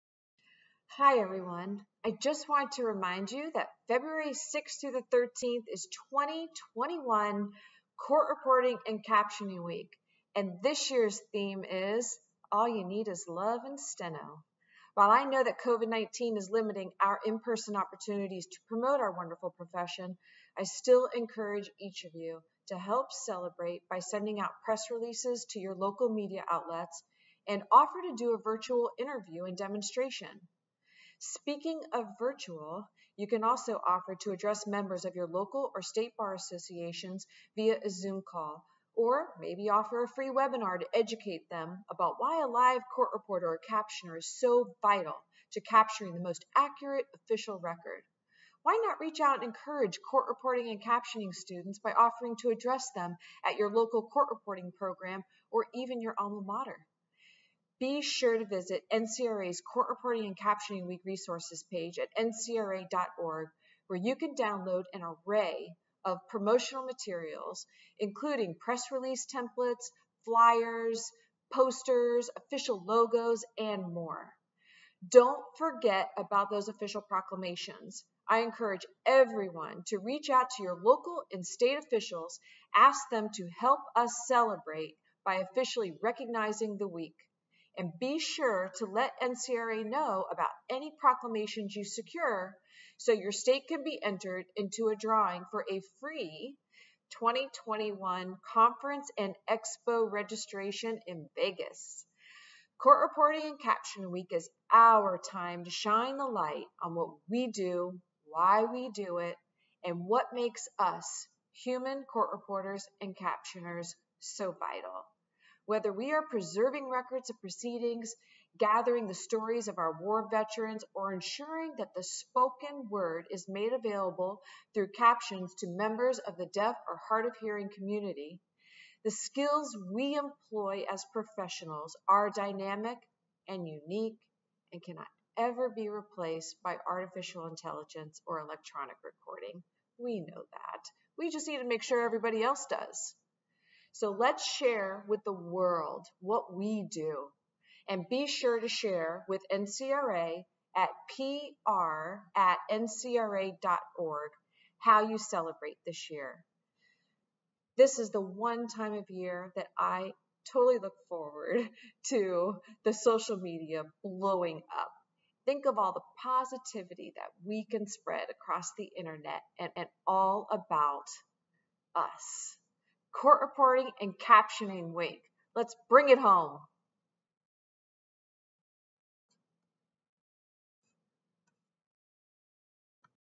President Christine Phipps
NCRA President Christine Phipps invites you to celebrate Court Reporting & Captioning Week!